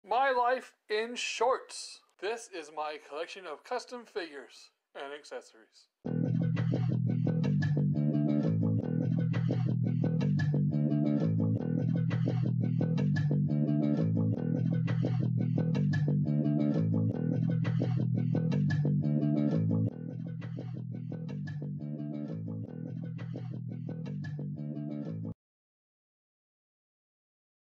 SHORTS #1... custom figures...
this is a short for Y.. this short is of my custom figures that IVE done, some on my channel and some before there was YT in my life. please check out my other videos here on IM KINDA SKETCHY.